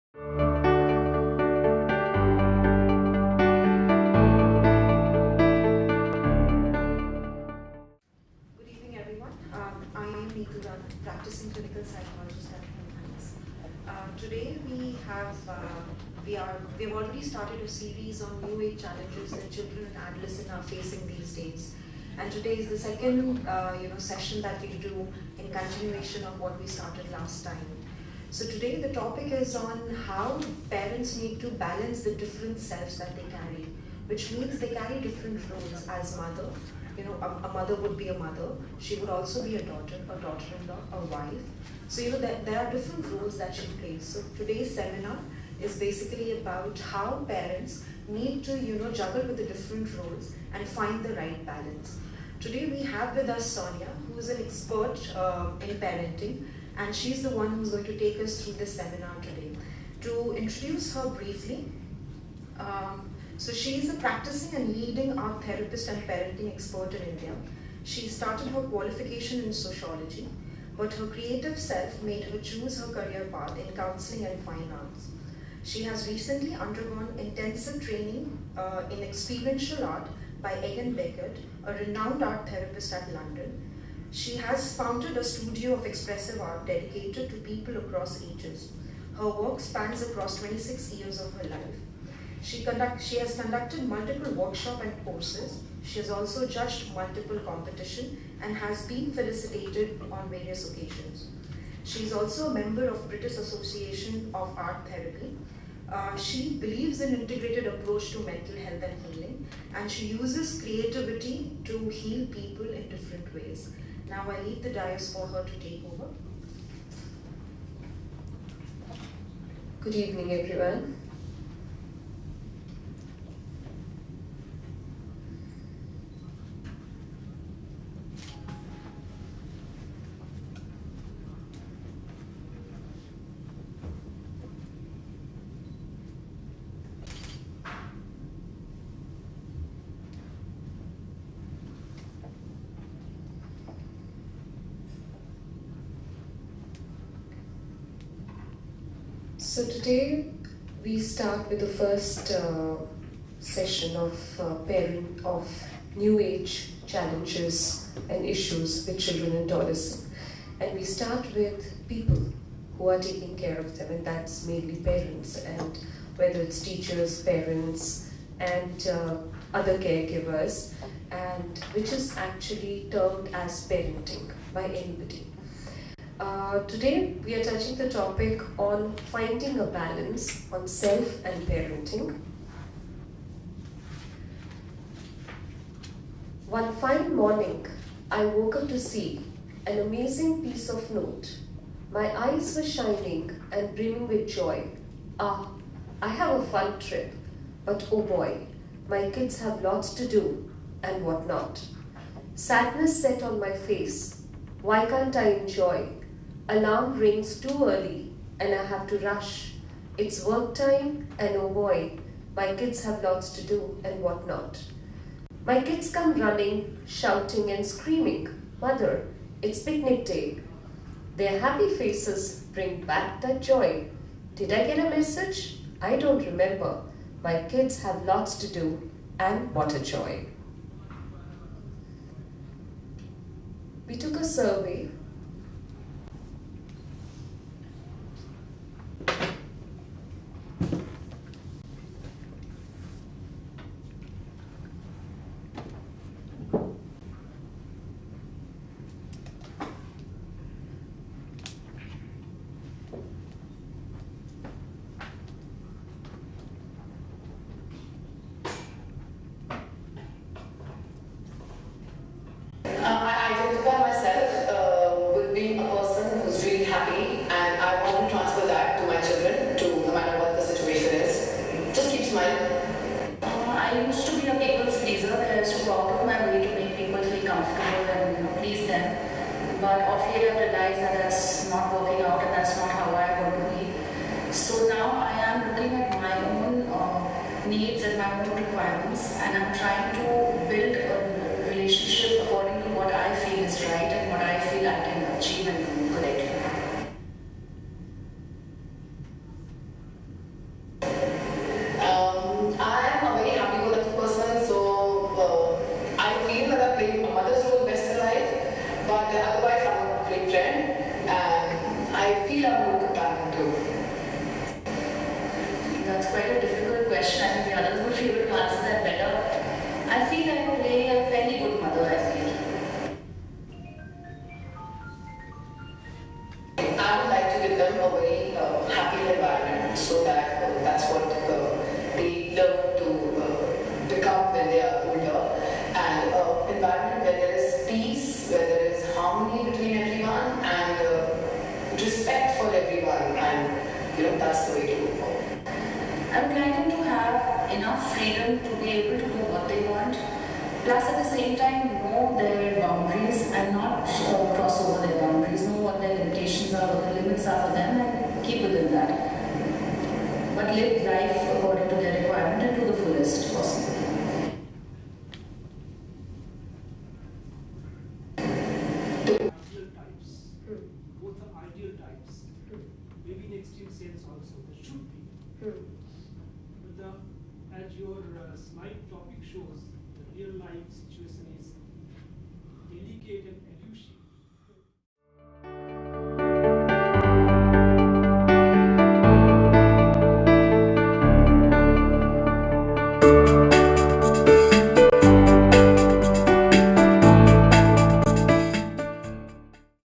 23 September 2016 Panel discussion on managing the complexities of modern-day p
Panel discussion on managing the complexities of modern-day parenting with personal and career growth. Panelists: Dr.Monalisa Palit, Dr Amit Sen, Yasmin Asghar, Seema Sahay, Dr Neetu Rana. Collab: Vimhans